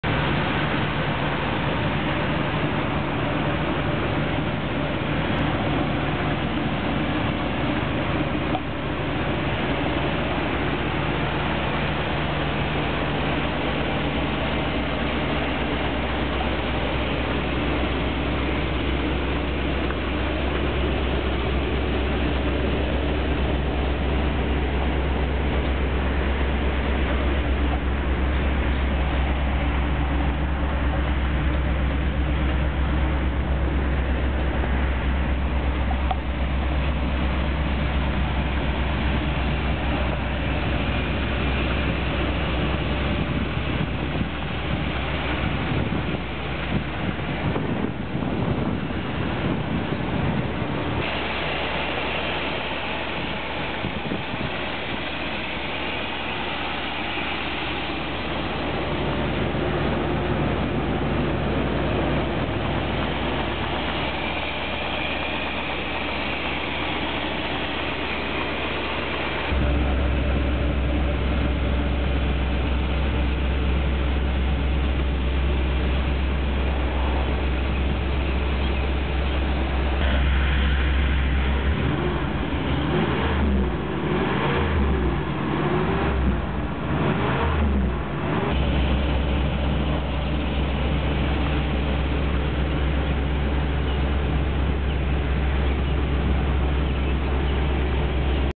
1976 Black Black Corvette Stingray Automatic
Numbers matching, 60,000 original miles, automatic, black leather interior, rally wheels, power steering, power brakes, power windows, tilt and tele steering column, and much more.